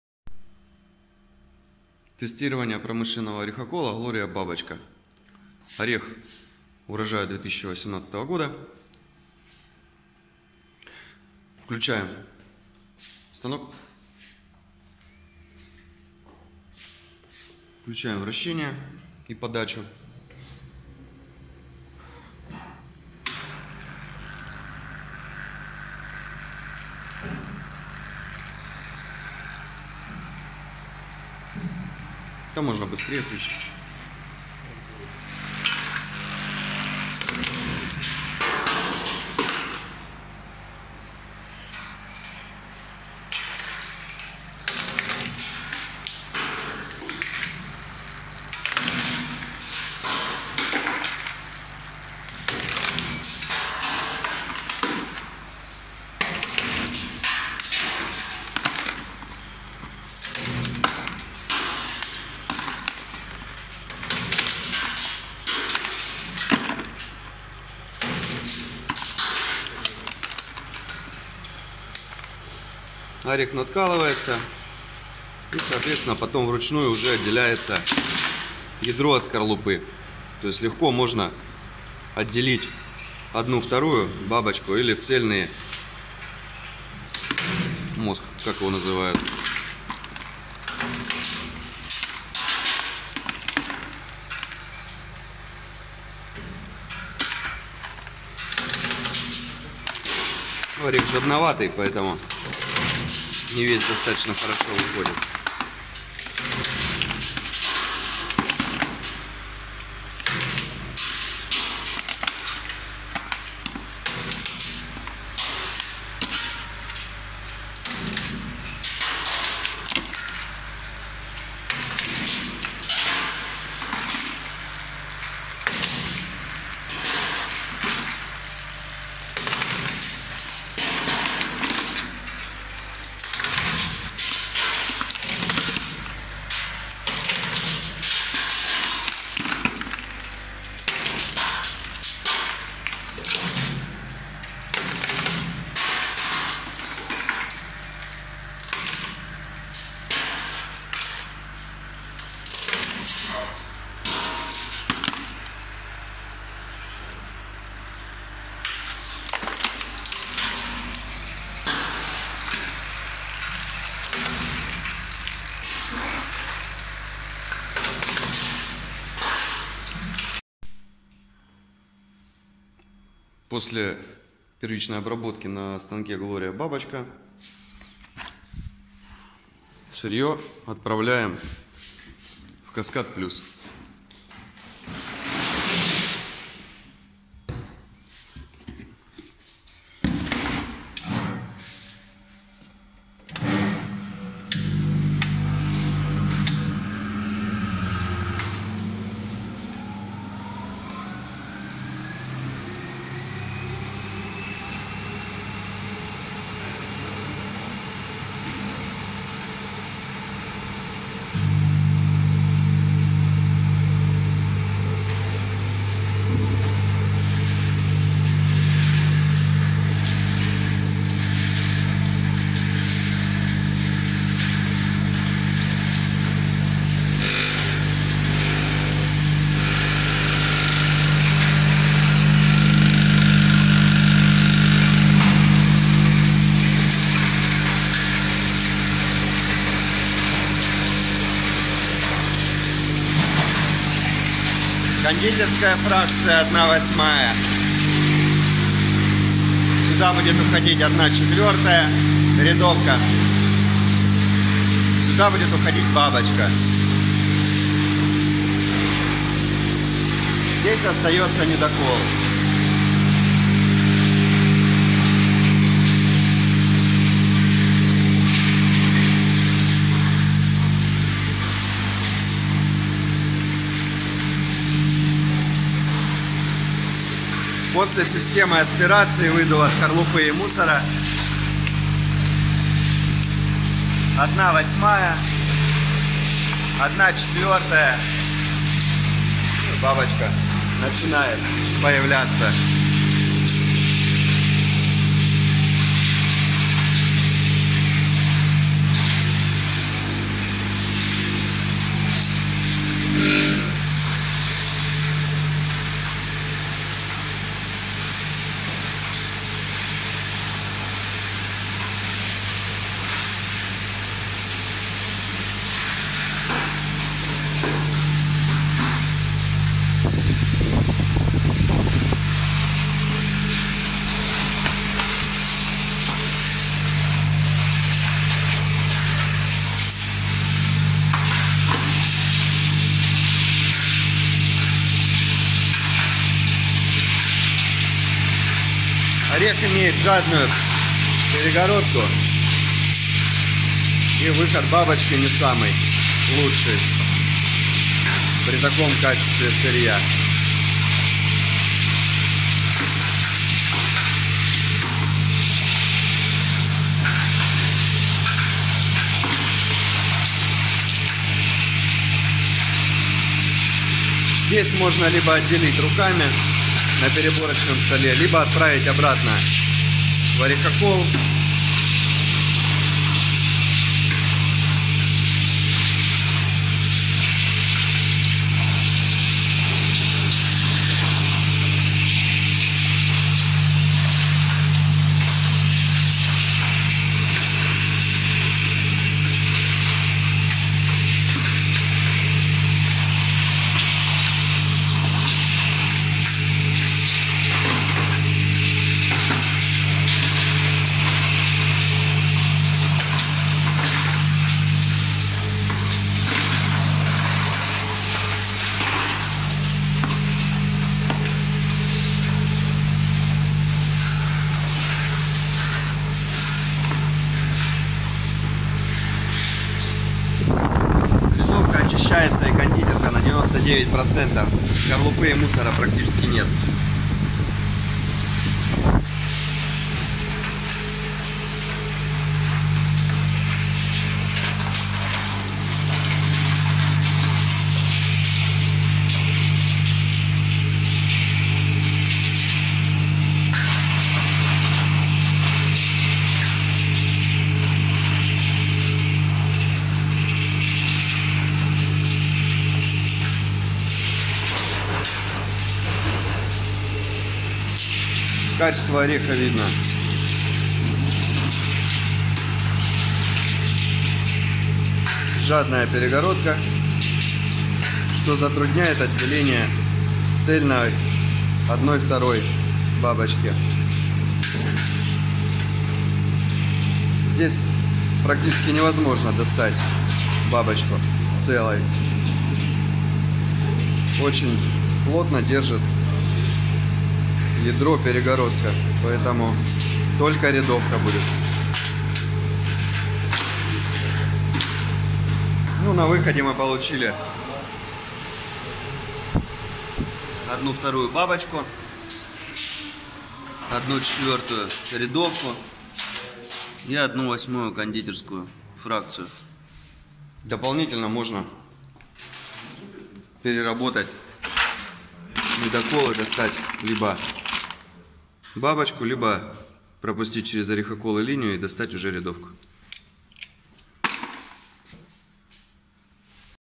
Глория и Каскад Плюс орехокол линия для грецких орехов
Демонстрация возможности совместного использования станка Глория Бабочка и линии Каскад Плюс.
Объёмы переработки, фракции сырья на выходе, количество 1/2 бабочки, рядовки и кондитерки, а также последующая ручная переборка напрямую зависит от качества исходного сырья цельного грецкого ореха! От толщины и фактуры скорлупы, от плотности (жадности перегородки), влажности ядра. Из жадного ореха много бабочки не выйдет, если вы в ручную или с помощью подручных средств не сможете достать из скорлупы целую 1/2 бабочку, то и не один станок этого вам не сделает!

Проводим бесплатное тестирование оборудования на вашем сырье!
Звоните прямо сейчас и запишитесь!

На нашем канале много видео демонстрации оборудования собственного производства!